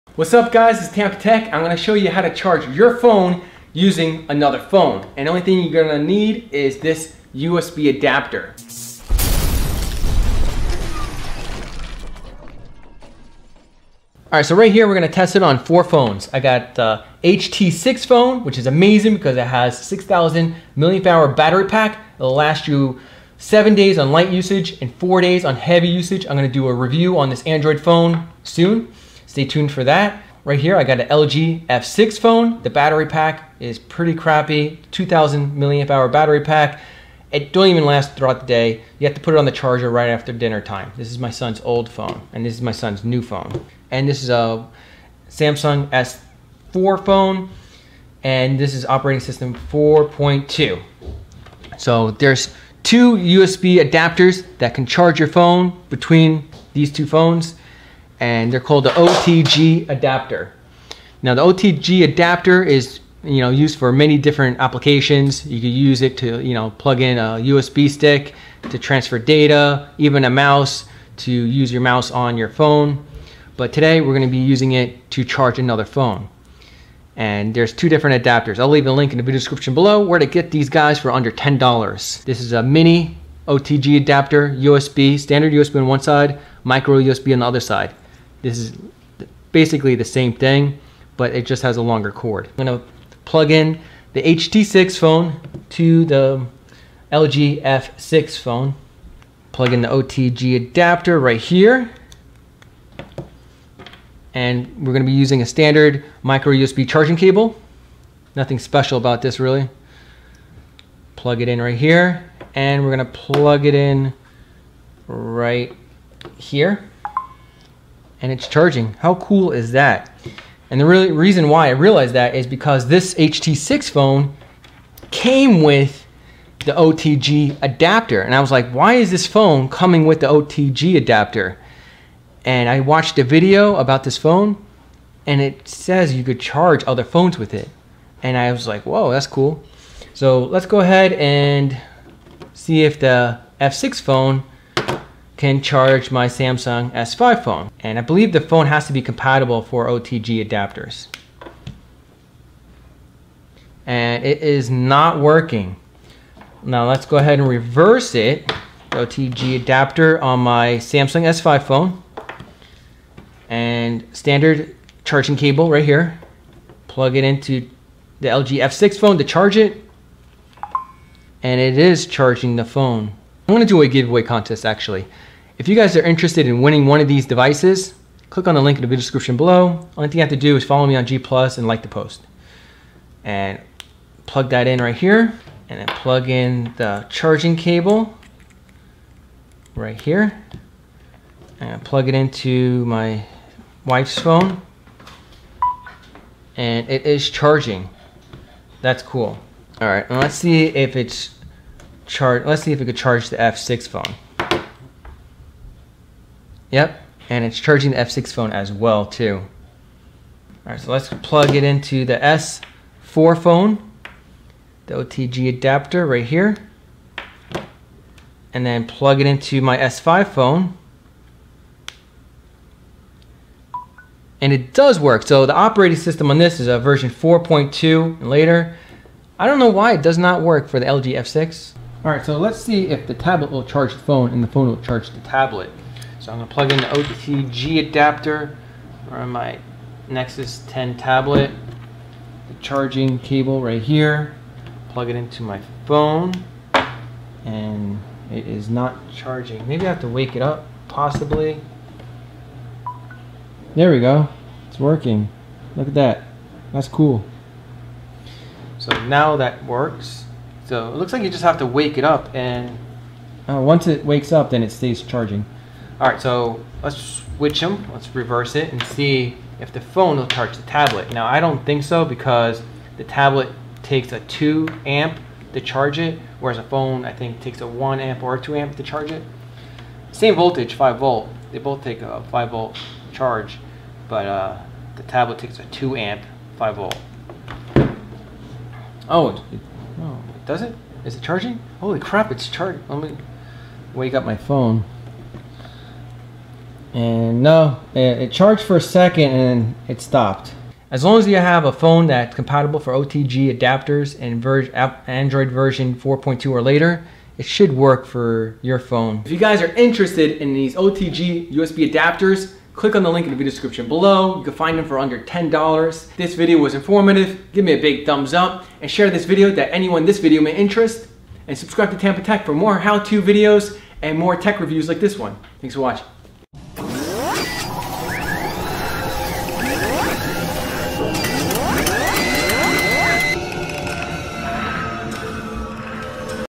How to charge your phone battery without AC charger using other phones
HOW TO CHARGE A BATTERY ON PHONES USING TABLETS OR PHONE, this works on android phones OS V4.2 5.1 Samsung S4 S5 S6 S7 NOTE3 NOTE 4, HTC, Nexus, G5 ect..
OTG MICRO USB TO USB ADAPTERS
SHORT OTG ADAPTER more below
IOS 9 iPhone 5 6 6S Plus iPad & Android